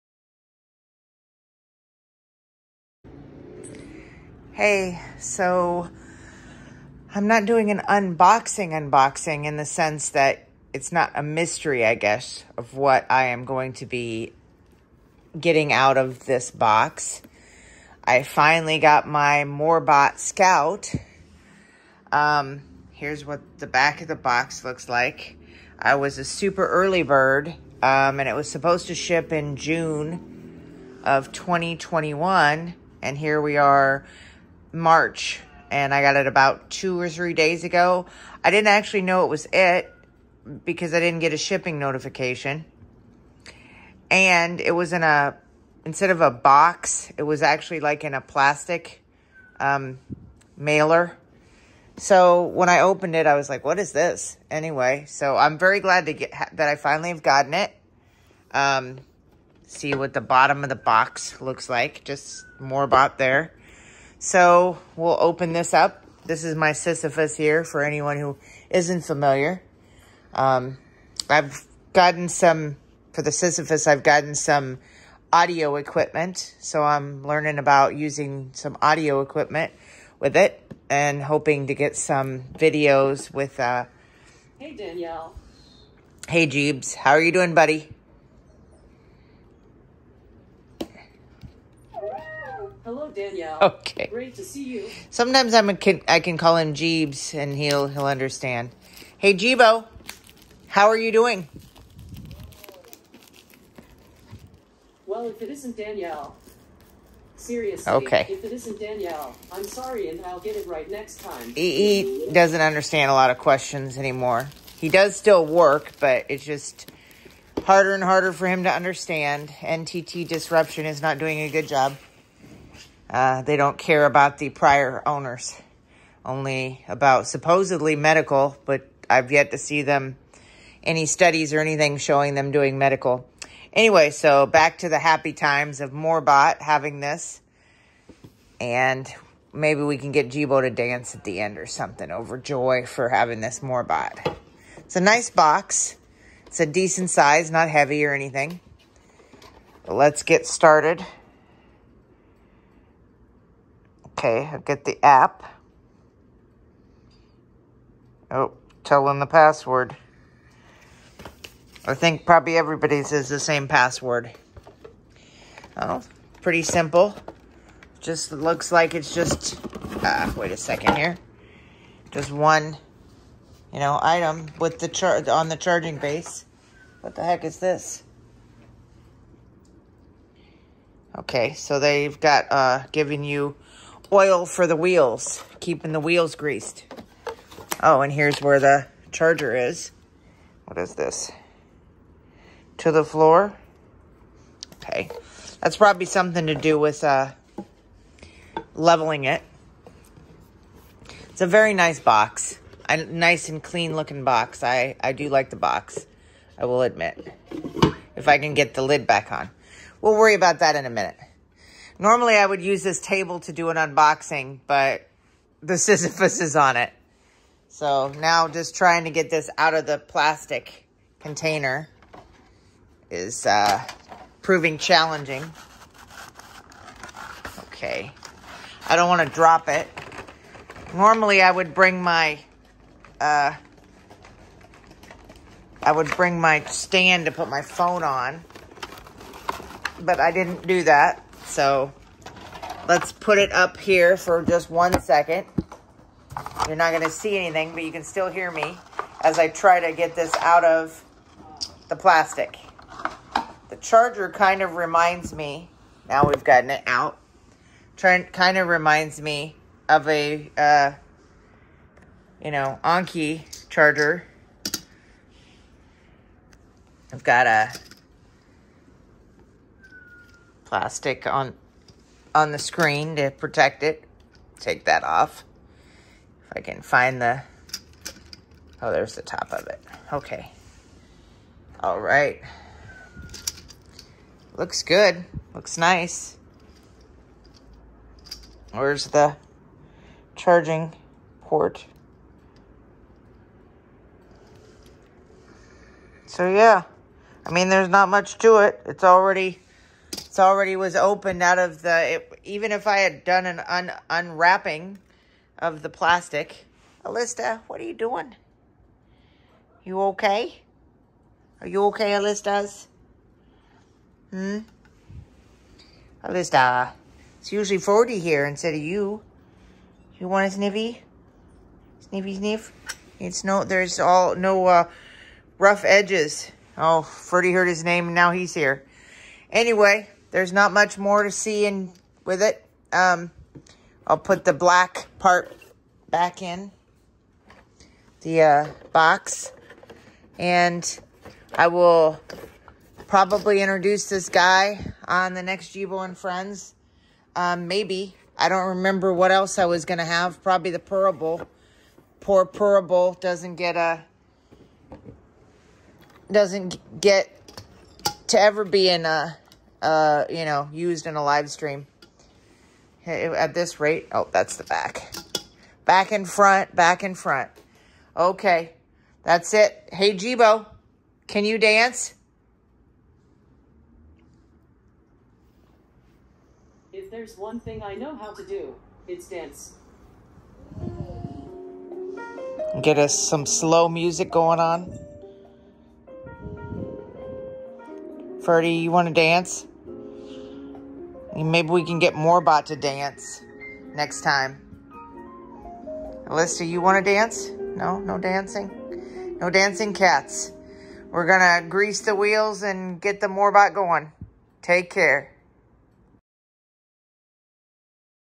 Moorebot Scout - Unboxing (Greasing The Wheels)
Last year, I was a super early bird backer for Moorebot Scout. That meant June 2021 shipping (per their Kickstarter). March 2022, I finally have my Scout robot!

See me unbox this bad boy. He's got omni-directional wheels (called Mecanum wheels), which is the main reason I got this robot. I can't wait to see him in action moving sideways in a future video!

If you'd like to get your own Moorebot Scout, hopefully faster than I did, you can now buy him